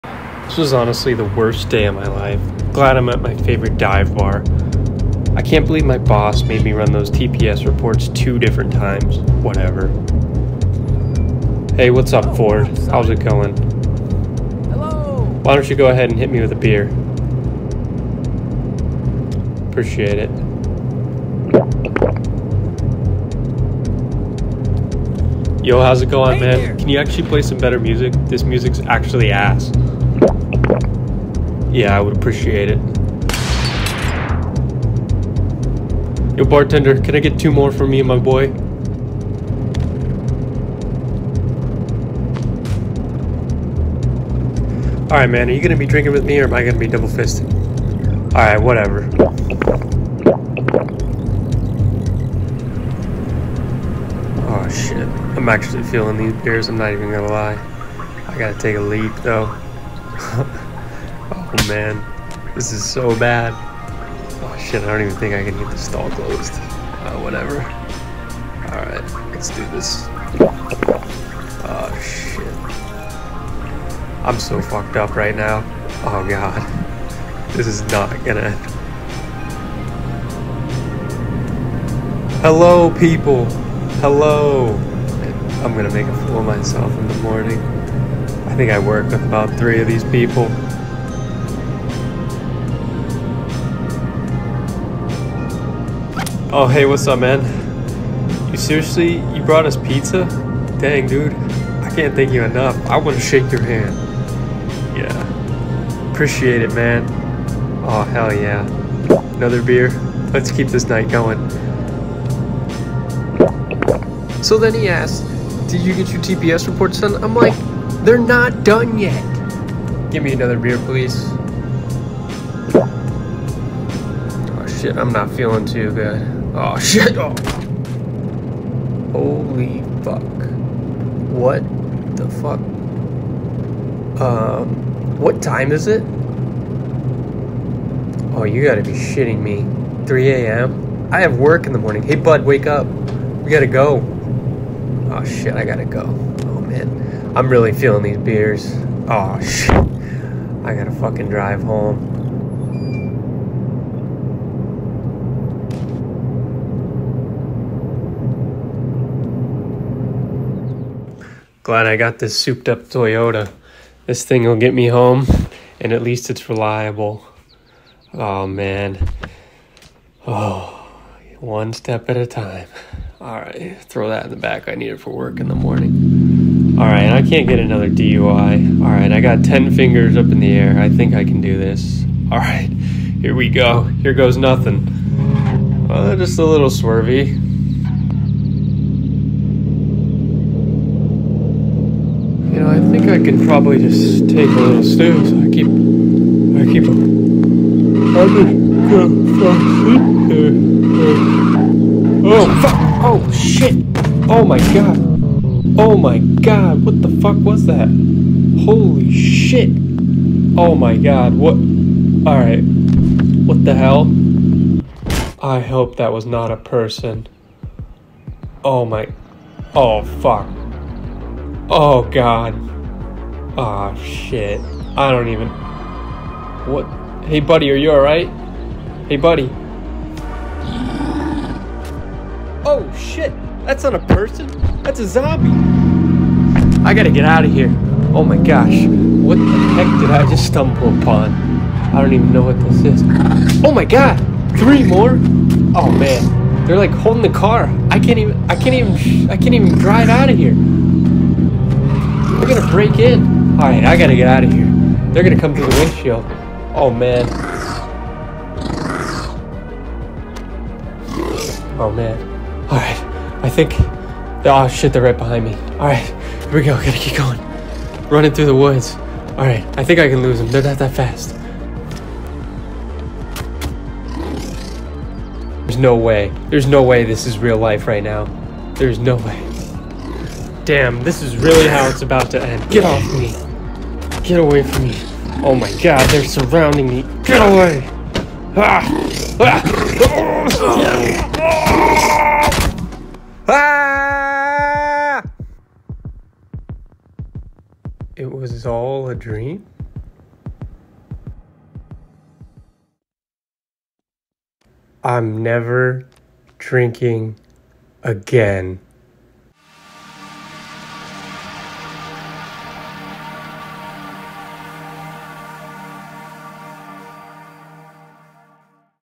Nightmare Road: A Zombie Encounter
A night drive like no other. Brace yourself as we navigate through darkness, uncertainty, and the relentless pursuit of the undead.